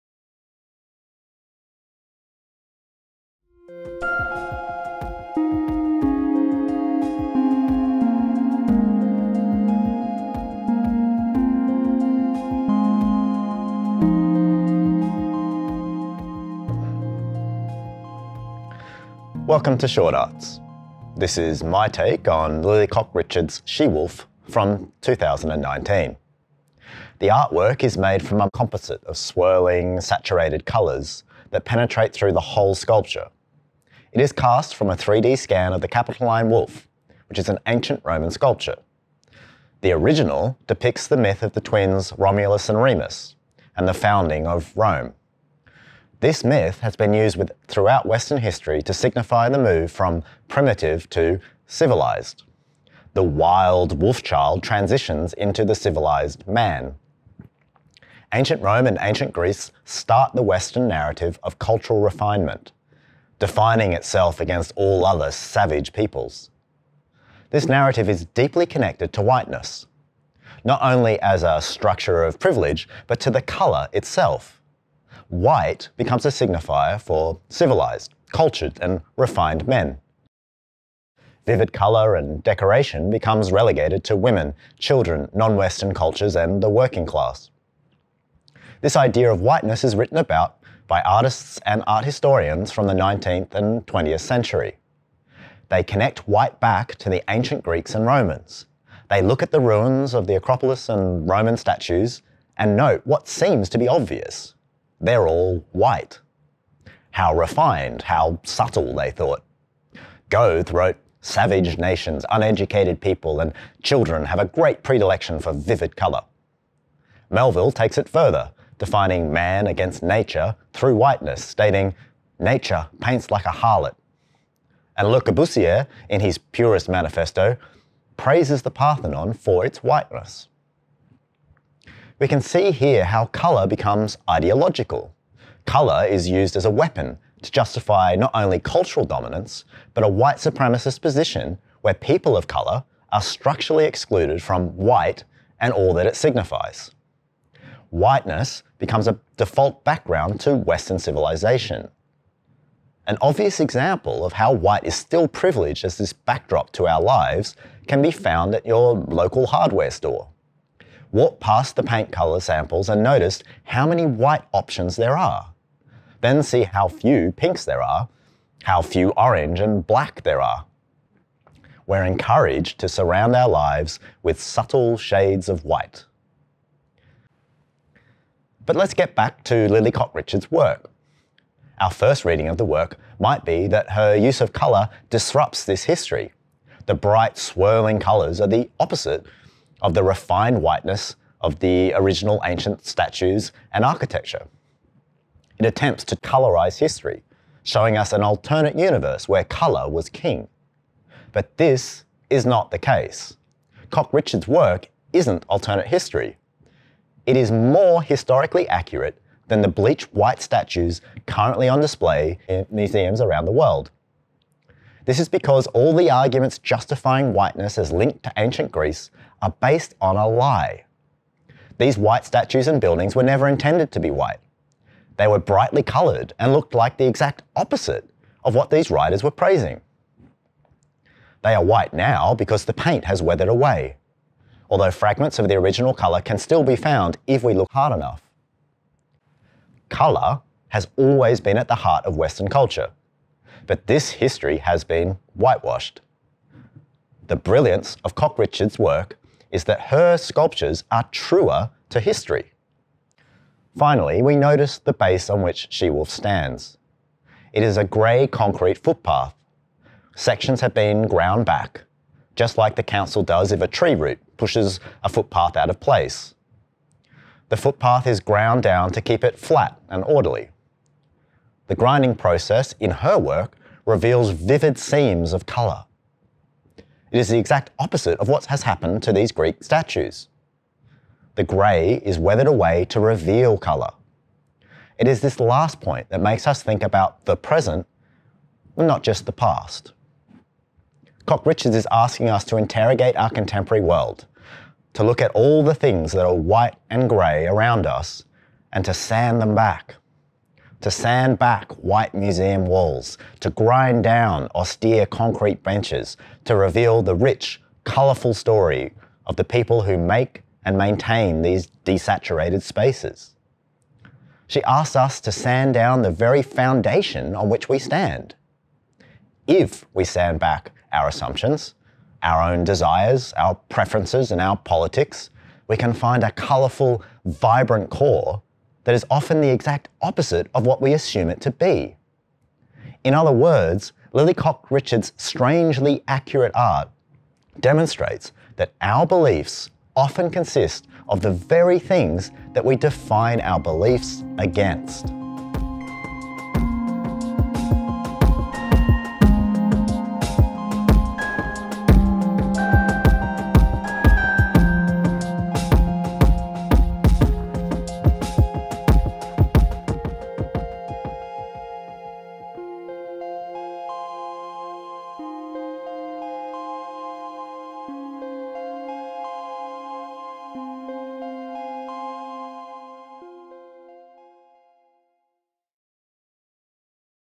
Contemporary Art Explained: Lily Cox-Richard
Themes: Whiteness, Contemporary Sculpture, Eurocentrism, Civilized /Savage

Description: In this video I discuss how Lily Cox-Richard’s colourful copy of The Capitoline Wolf encourages us to think about whiteness not only as a colour, but as an ideology. The superiority of whiteness has been justified through the white ruins of Ancient Greek and Roman statues and architecture. However this is not accurate, as these were originally intensely coloured, but the paint has weathered away. Cox-Richard’s strangely accurate art shows the lie of this narrative, and shows us colour has always been at the heart of Western culture.

Educational Resource: A list of discussion questions, a studio activity, definition of key terms and artist quotes can be found on my